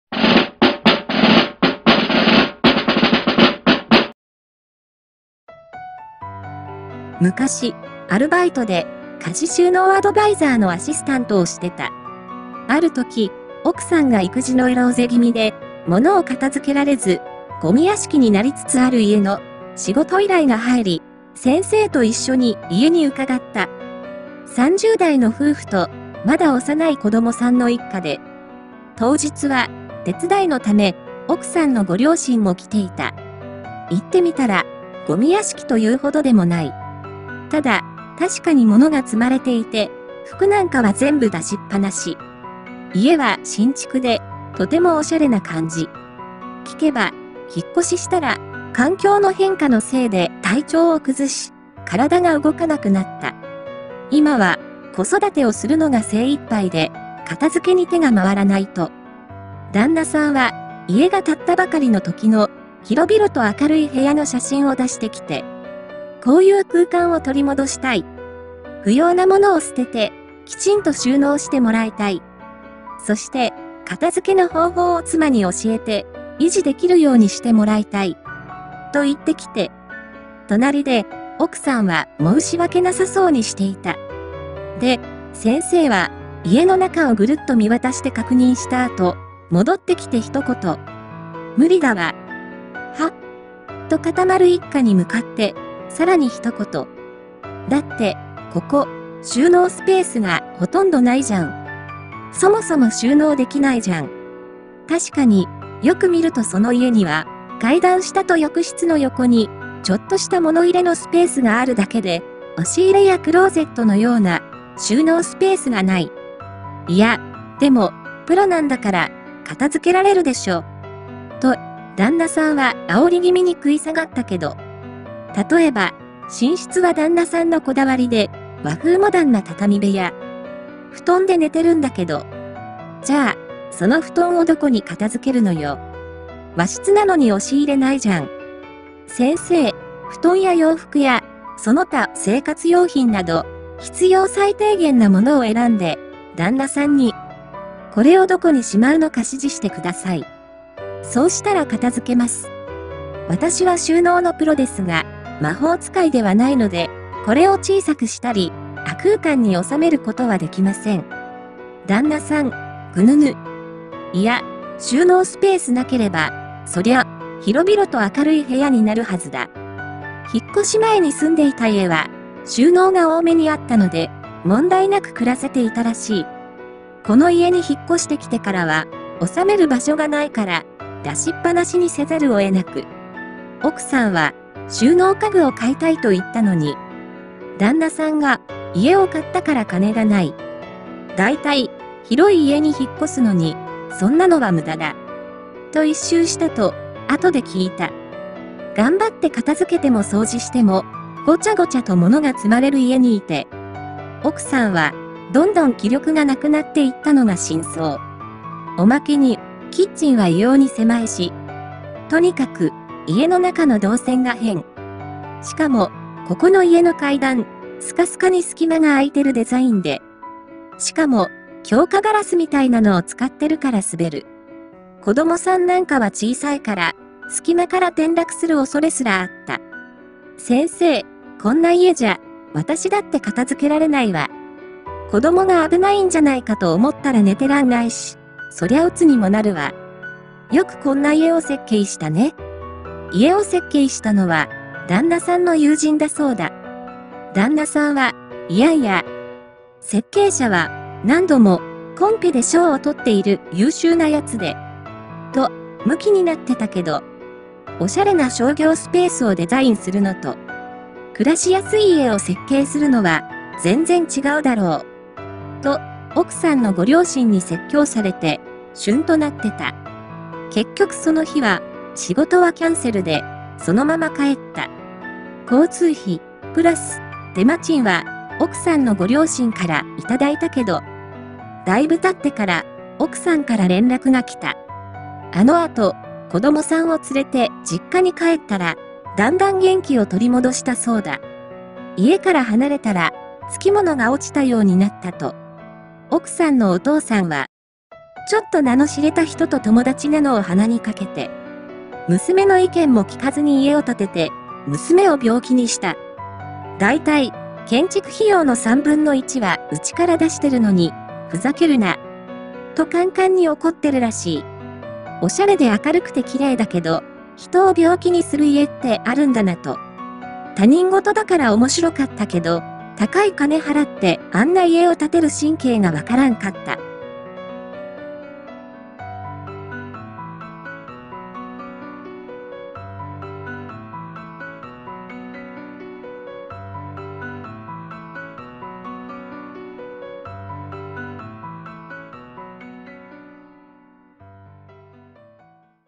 旦那「妻が片付けができなくて」妻「・・・」旦那「不要なものを捨ててきちんと収納してもらいたい。そして、その方法を妻に教えてほしい」先生「こりゃ無理だわｗ」旦那「は？！」【修羅場】
DOVA-SYNDROME